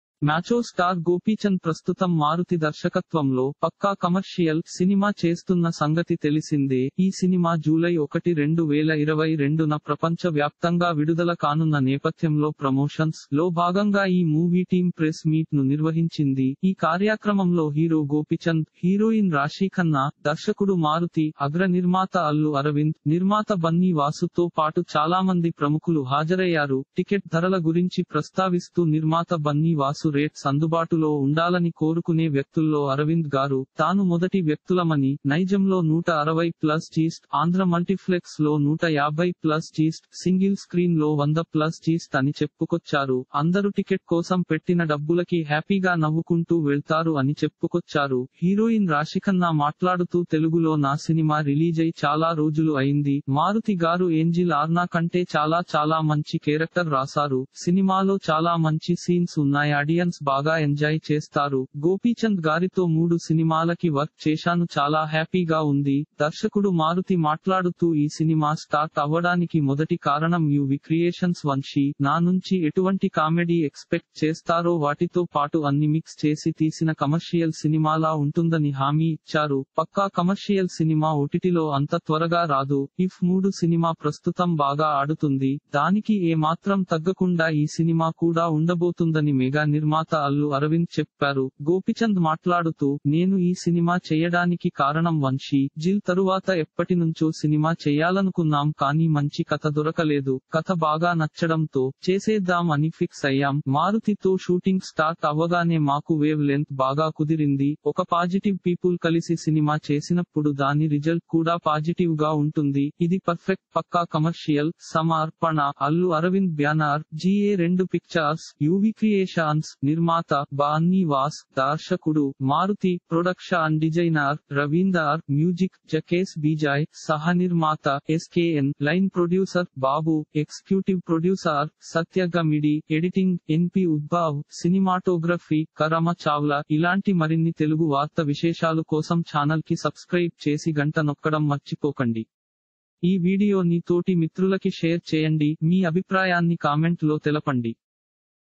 Allu Arvind - పక్కా కమర్షియల్, టిక్కెట్ల ధ‌రలు పై సృష్ట‌త ఇచ్చిన అల్లుఅర‌వింద్, నిర్మాత బ‌న్నీవాస
మ్యాచో స్టార్ గోపీచంద్ ప్రస్తుతం మారుతి దర్శకత్వంలో "పక్కా కమర్షియల్" సినిమా చేస్తున్న సంగతి తెలిసిందే. ఈ సినిమా జూలై 1, 2022న ప్రపంచ వ్యాప్తంగా విడుదల కానున్న నేపథ్యంలో ప్రమోషన్స్’లో భాగంగా ఈ మూవీ టీం ప్రెస్ మీట్ ను నిర్వహించింది.  ఈ కార్యాక్రమంలో హీరో గోపిచంద్, హీరోయిన్ రాశీఖన్నా, దర్శకుడు మారుతి, అగ్ర నిర్మాత అల్లు అరవింద్, నిర్మాత బన్నీ వాసు తో పాటు చాలామంది ప్రముఖులు హాజరయ్యారు. టికెట్ ధరల గురించి ప్రస్తావిస్తూ నిర్మాత బన్నీ వాసు రేట్స్ అందుబాటులో ఉండాలని కోరుకునే వ్యక్తుల్లో అరవింద్ గారు ,తాను మొదటి వ్యక్తులమని. నైజంలో 160+gst, ఆంధ్ర మల్టిఫ్లెక్స్ లో 150+gst, సింగిల్ స్క్రీన్ లో 100+gst అని చెప్పుకొచ్చారు. అందరు టికెట్ కోసం పెట్టిన డబ్బులకి హ్యాపీగా నవ్వుకుంటూ వెళ్తారు అని చెప్పుకొచ్చారు.

హీరోయిన్ రాశిఖన్నా మాట్లాడుతూ తెలుగులో నా సినిమా రిలీజై చాలా రోజులు అయింది, మారుతి గారు ఏంజిల్ ఆర్నా కంటే చాలా చాలా మంచి కేరక్టర్ రాసారు. సినిమాలో చాలా మంచి  సీన్స్  ఉన్నాయ్ ఆడియన్స్ బాగా ఎంజాయ్ చేస్తారు. గోపీచంద్ గారితో మూడు సినిమాలకి వర్క్ చేశాను చాలా హ్యాపీ గా ఉంది. దర్శకుడు మారుతి మాట్లాడుతూ ఈ సినిమా స్టార్ట్ అవ్వడానికి మొదటి కారణం యు.వి క్రియేషన్స్ వంశీ. నా నుంచి ఎటువంటి కామెడీ ఎక్స్పెక్ట్ చేస్తారో వాటితో పాటు అన్ని మిక్స్ చేసి తీసిన కమర్షియల్ సినిమాలా ఉంటుందని హామీ ఇచ్చారు. పక్కా కమర్షియల్ సినిమా ఓటిటిలో అంత త్వరగా రాదు, f3 సినిమా ప్రస్తుతం బాగా ఆడుతుంది, దానికి ఏ మాత్రం తగ్గకుండా ఈ సినిమా కూడా ఉండబోతుందని మెగా నిర్మాత అల్లు అరవింద్ చెప్పారు.

గోపిచంద్ మాట్లాడుతూ..నేను ఈ సినిమా చెయ్యడానికి  కారణం వంశీ, జిల్ తరువాత ఎప్పటినుంచో సినిమా చెయ్యాలనుకున్నాం కానీ మంచి కథ దొరకలేదు. కథ బాగా నచ్చడంతో, చేసేద్దాం అని ఫిక్స్ అయ్యాం. మారుతి తో షూటింగ్ స్టార్ట్ అవ్వగానే మాకు వేవ్ లెన్త్ బాగా కుదిరింది. ఒక పాజిటివ్ పీపుల్ కలిసి సినిమా చేసినప్పుడు దాని రిజల్ట్ కూడా పాజిటివ్ గా ఉంటుంది. ఇది పర్ఫెక్ట్ పక్కా కమర్షియల్.